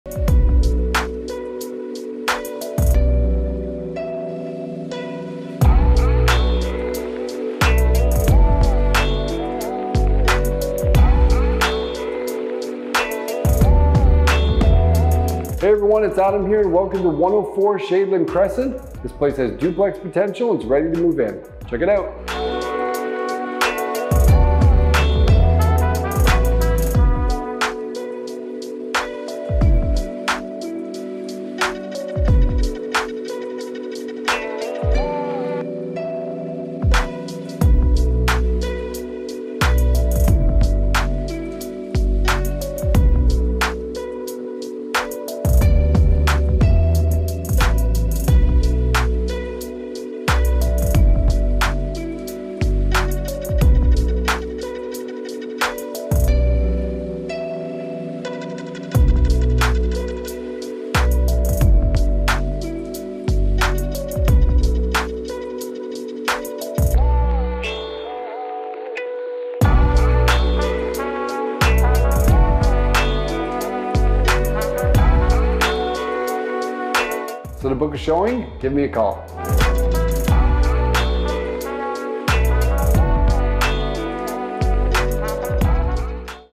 104 Shadeland Crescent, Kitchener
Golden opportunity with duplex close to completion! This three bedroom and two full bath home is perfect for those looking to get into the market, looking for onsite rental income to finance their mortgage, or looking to purchase a possible duplex with excellent income potential. Lots of updates! Freshly painted neutral colours throughout (2023). Upper bathroom shower has been re tiled (2023) along with new light fixtures. The spacious main floor kitchen with freshly painted cupboards, new countertops & faucet (2023). Open concept living room and dining room are perfect for entertaining. New carpet on stairs (2023). Upstairs you will find a large primary bedroom and good sized second and third bedrooms. The basement features a potential bachelor apartment, where you will find a separate entrance, full kitchen, full bathroom, newer furnace and AC, as well as a new 200 Amp panel accompanied by an ESA certificate showing upgraded wiring in all rooms. Four appliances will be included. Other updates include: Roof (2022), eavestroughs/soffits (2019) plus a brand new rented hot water heater (2023). There is also a spacious storage shed and a very deep, private backyard. Close to parks, trails, schools, community centre, university express bus line, shopping and so much more! Quick possession available. Book your showing today!